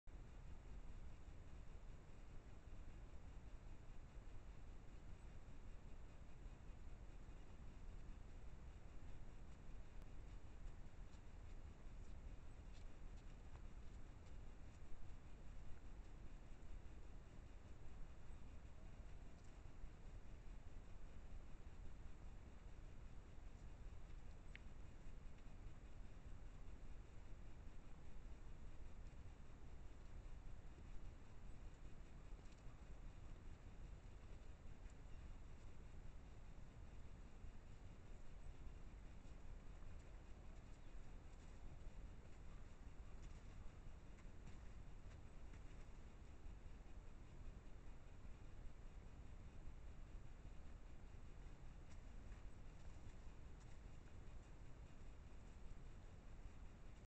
Maine North Woods Trap Camera--Deer, Using the Snowblowed Lane
Deer using the snowblowed lane.
It is nice to see the deer using the lane we snow blowed for them.